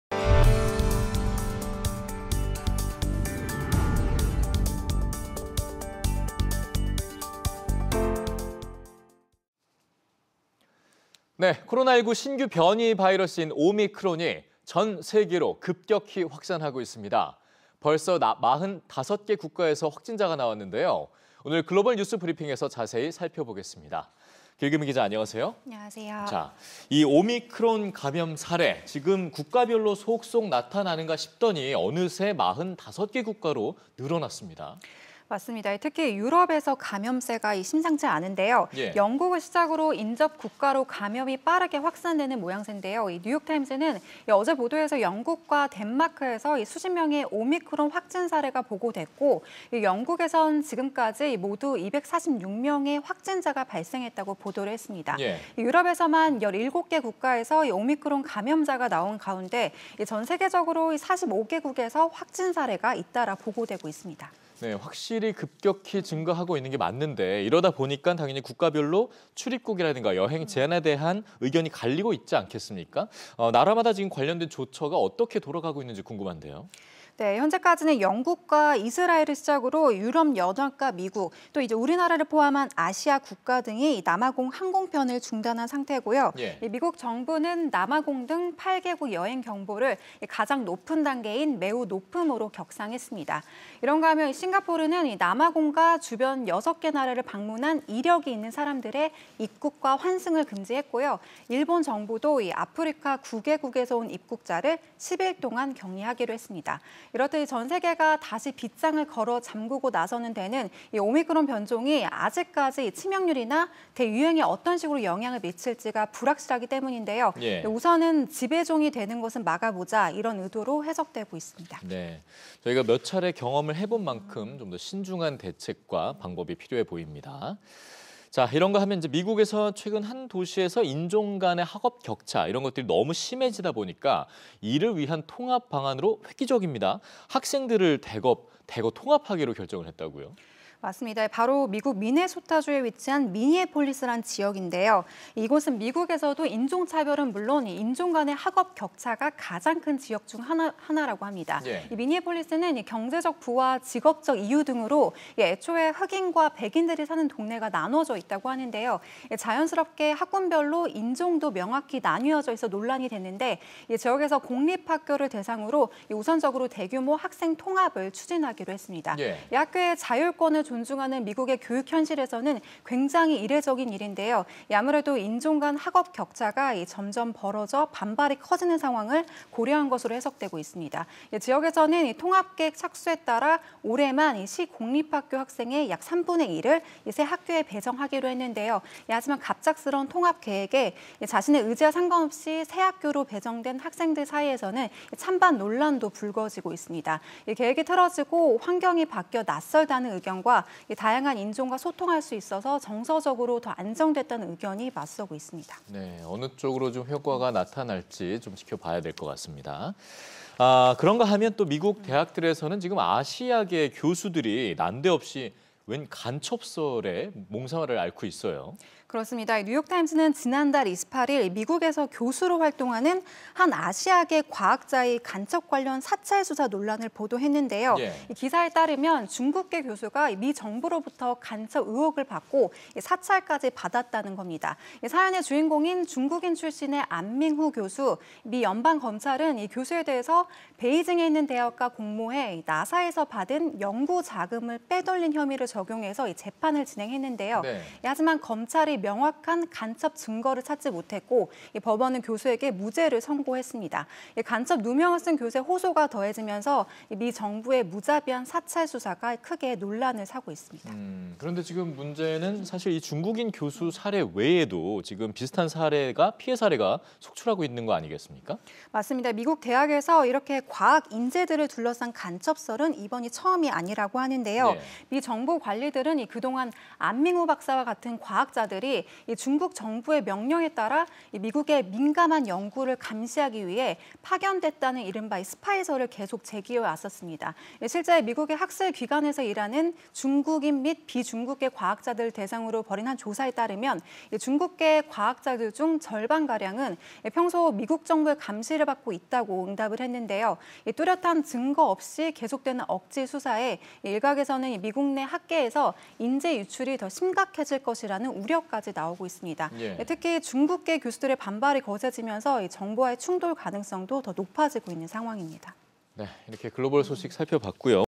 2021. 12. 06. 오미크론, 45개국서 감염 보고‥유럽도 '비상'
[EBS 저녁뉴스]

코로나19 신규 변이 바이러스인 오미크론이 전 세계로 급격히 확산하고 있습니다.

벌써 45개 국가에서 확진자가 나왔는데요.

오늘 글로벌 뉴스 브리핑에서 자세히 살펴보겠습니다.

길금희 기자, 국가별로 첫 감염 사례가 속속 나타나는가 싶더니 순식간에 45개국까지 확산됐네요?

길금희 기자
특히 유럽에서 감염세가 심상치 않습니다.

영국을 시작으로 인접 국가로 감염이 빠르게 확산되는 모양샌데요.

뉴욕타임스는 어제 보도에서 영국과 덴마크에서 수십 명의 오미크론 확진 사례가 보고됐고, 영국에선 지금까지 모두 246명의 확진자가 발생했다고 보도했습니다.

유럽에서만 17개 국가에서 오미크론 감염자가 나온 가운데 전 세계적으로 45개국에서 확진 사례가 보고됐습니다.

국가별로 출입국이나 여행 제한을 두고도 의견이 갈리고 있는데, 나라마다 관련한 조처는 어떻게 돌아가고 있습니까

길금희 기자
현재 영국과 이스라엘을 시작으로 유럽연합(EU)과 미국, 또 우리나라를 포함한 아시아 국가 등이 남아공 항공편을 중단했고요.

미국 정부는 남아공 등 8개국 여행경보를 가장 높은 단계인 '매우 높음'으로 격상했습니다.

이런가 하면 싱가포르는 남아공과 주변 6개 나라를 방문한 이력이 있는 사람들의 입국과 환승을 금지했고요, 일본 정부도 아프리카 9개국에서 온 입국자를 10일 동안 격리하기로 했습니다.

이렇듯 전 세계가 다시 빗장을 걸어 잠그고 나서는 데는 오미크론 변종이 아직까지 치명률이나 대유행에 어떤 식으로 영향을 미칠지가 불확실하기 때문인데요.

우선은 "지배종이 되는 것은 막아보자" 이런 의도로 해석되고 있습니다.

이런가 하면 미국에선 최근 한 도시에서 인종 간 학업 격차가 너무 심해지다 보니까 이를 줄이기 위한 방안으로 학생들을 대거 통합하기로 결정했다고요?

길금희 기자
바로 미국 미네소타주에 위치한 미니애폴리스란 지역인데요.

미국에서도 인종차별은 물론, 인종 간의 학업 격차가 가장 큰 지역 중 한곳입니다.

미니애폴리스는 경제적 부와 직업적 이유 등으로 애초 흑인과 백인들이 사는 동네가 나눠져 있는데요.

자연스럽게 학군별로 인종도 명확히 나뉘어져 논란이 됐는데 지역에서 공립학교를 대상으로 대규모 학생 통합을 추진했습니다.

학교의 자율권을 존중하는 미국의 교육 현실에선, 굉장히 이례적인 일인데요.

아무래도 인종 간 학업 격차가 점점 벌어져 반발이 커지는 상황을 고려한 것으로 보입니다.

지역에선 통합 계획에 착수에 따라 올해만 시 공립학교 학생의 약 3분의 1을 새 학교에 배정했는데요.

하지만 갑작스런 통합계획에 자신의 의지와 상관없이 새학교로 배정된 학생들 사이 찬반 논란도 불거지고 있습니다.

계획이 틀어지고 환경이 바뀌어 낯설다는 의견과 다양한 인종과 소통할 수 있어 정서적으로 더 안정됐다는 의견이 맞서고 있습니다.

미국 대학들에선 아시아계 교수들이 난데없는 간첩설에 몸살을 앓고 있다고요?

길금희 기자
뉴욕타임스는 지난달 28일, 미국에서 교수로 활동하는 한 아시아계 과학자의 간첩 관련 사찰 수사 논란을 보도했습니다.

기사에 따르면, 중국계 교수가 미 정부로부터 간첩 의혹을 받고 사찰까지 받았다는 건데요.

사연의 주인공인 중국인 출신의 Anming Hu(안밍 후) 교수, 미 연방검찰은 이 교수에 대해 베이징에 있는 대학과 공모해 NASA에서 받은 연구 자금을 빼돌린 혐의를 적용해 재판을 진행했습니다.

하지만 검찰이 명확한 간첩 증거를 찾지 못했고, 법원은 교수에게 무죄를 선고했는데요, 간첩 누명을 쓴 교수의 호소가 더해지며 미 정부의 무자비한 사찰 수사가 크게 논란을 사고 있습니다.

이번 중국인 교수 사례 외에도 비슷한 피해 사례가 계속 나오고 있다죠?

길금희 기자
미국 대학에서 이렇게 과학 인재들을 둘러싼 간첩설은 이번이 처음이 아닙니다.

미 정부 관리들은 그동안 안밍 후 박사와 같은 과학자들이 중국 정부의 명령에 따라 미국의 민감한 연구를 감시하기 위해 파견됐다는 이른바 '스파이'설을 계속 제기해왔는데요.

실제 미국의 학술기관에서 일하는 중국인 및 비중국계 과학자들을 대상으로 벌인 한 조사에 따르면, 중국계 과학자들 중 절반 가량은 평소 미국 정부의 감시를 받고 있다고 답했습니다.

뚜렷한 증거 없이 계속되는 억지 수사에 일각에선 미국 내 학계에서 인재 유출이 더 심각해질 것이란 우려도 나오고 있는데요.

특히 중국계 교수들의 반발이 거세지면서 정부와의 충돌 가능성도 더 높아지고 있습니다.